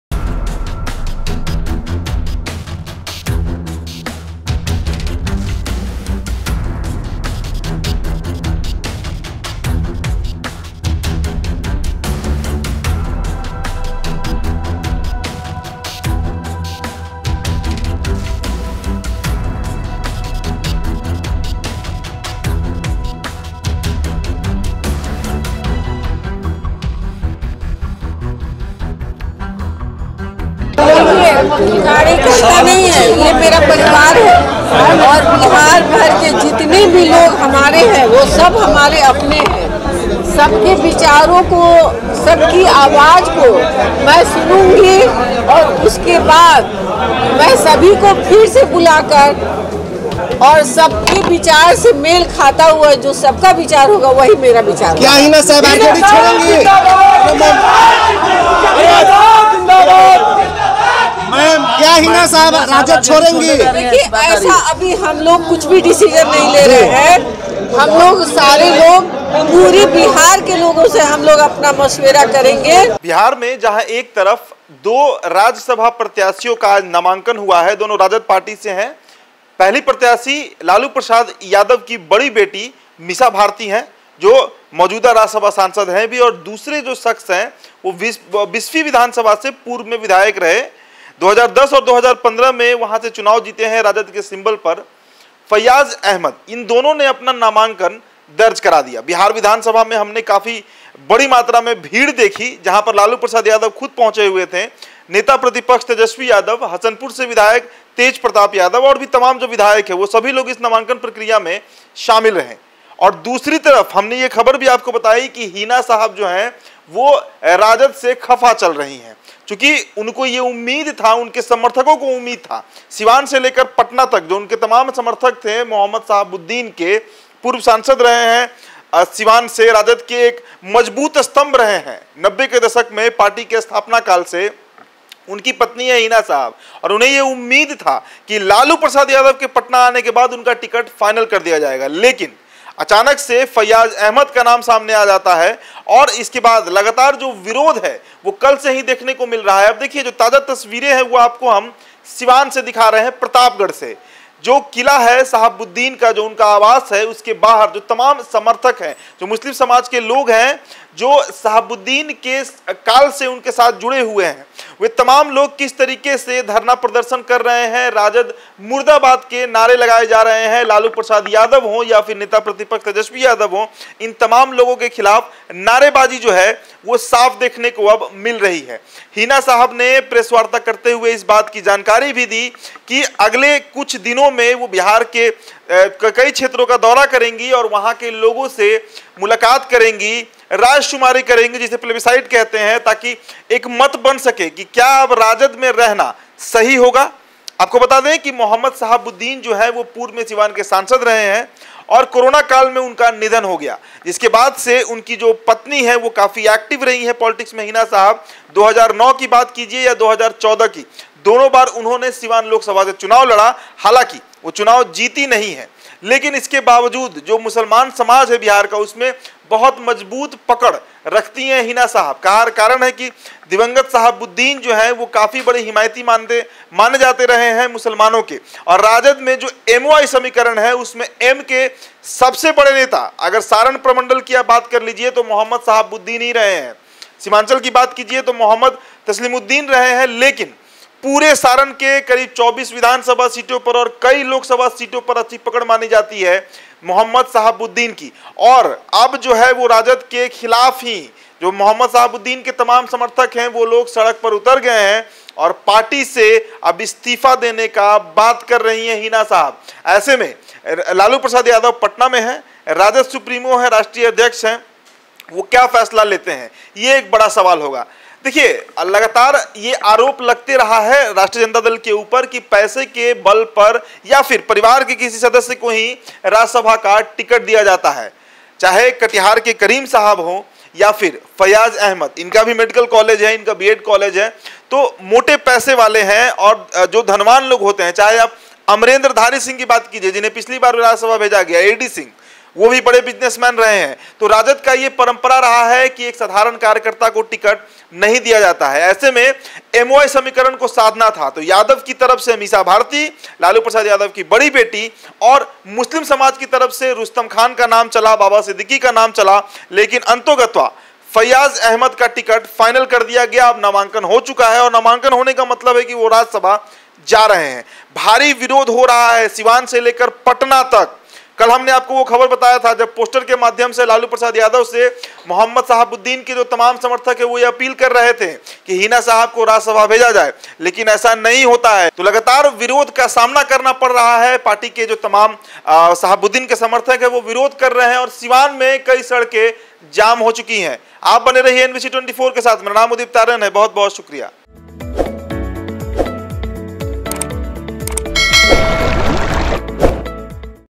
Siwan में Lalu Yadav के खिलाफ मुसलमान लगा रहे मुर्दाबाद के नारे,Hina Sahab को राज्यसभा टिकट न देने..
Siwan में Lalu Yadav के खिलाफ मुसलमान लगा रहे मुर्दाबाद के नारे, Hina Sahab को राज्यसभा टिकट न देने से है खफा...